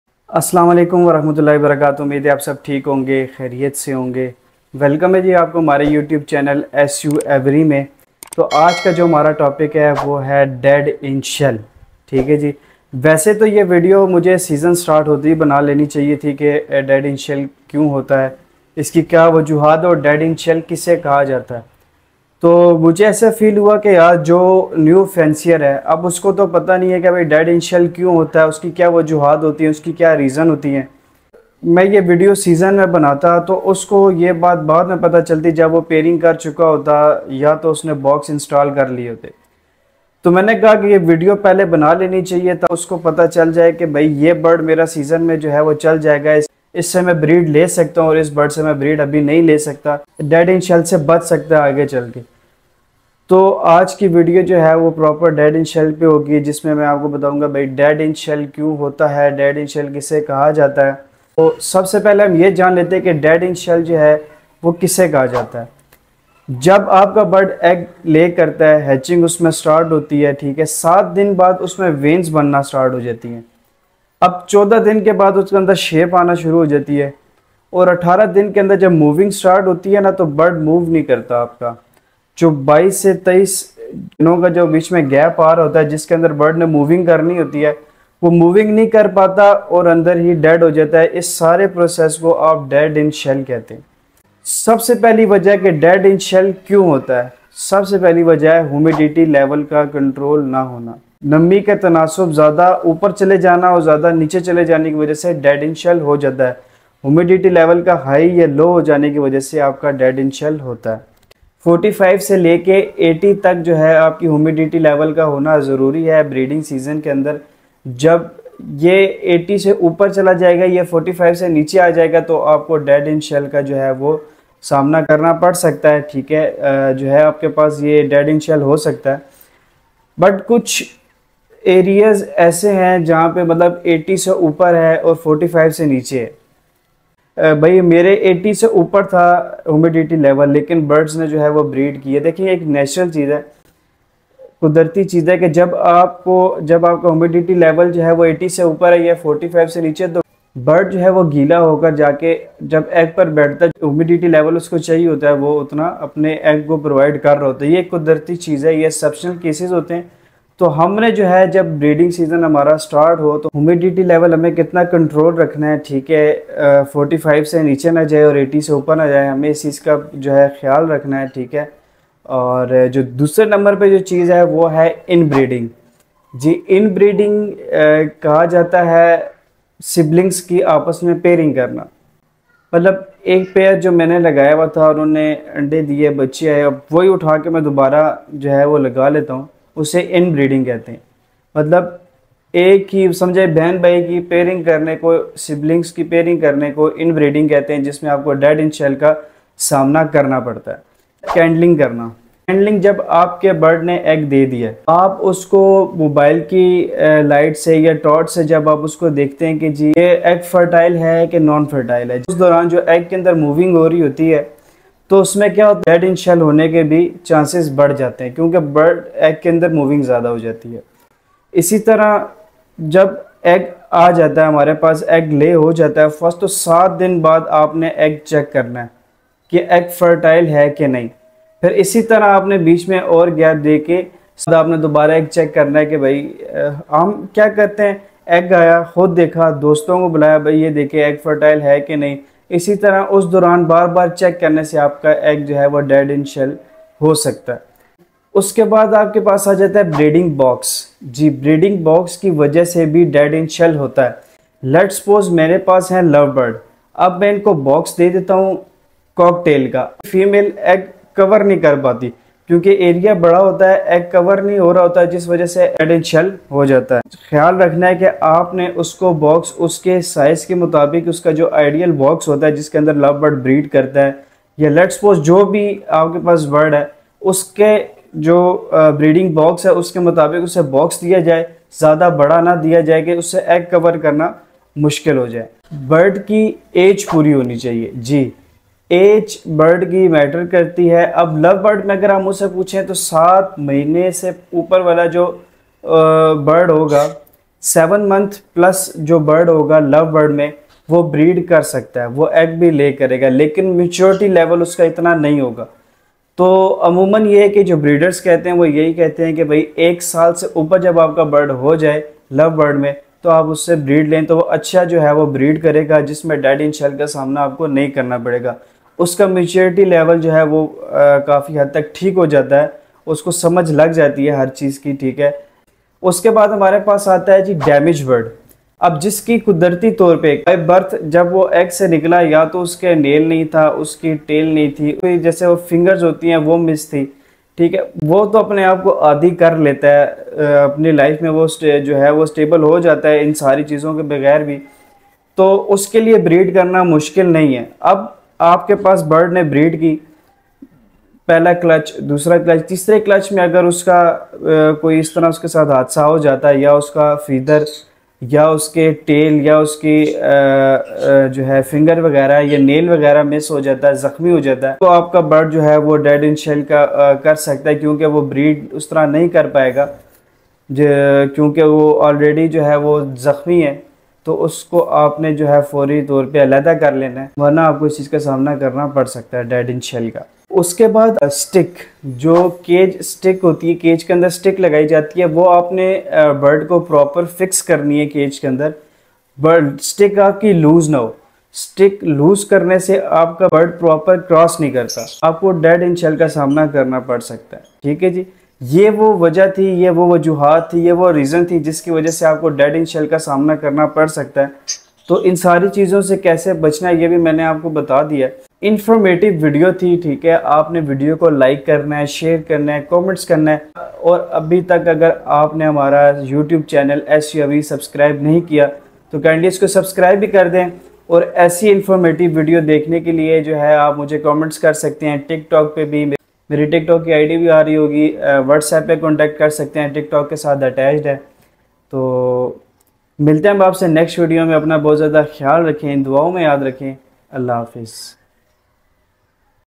Dead In Shell | Lovebirds dead in shell
- Titel : Dead in shell egg

- In this video: Why would that be dead in shell egg ?

1. Humidity level high and low  ￼￼
2. Inbreeding siblings pairing ￼
3. Candling  over and over again
4. Breeding box
5. Age in compelet
6. Damage birds
7. Cage sticks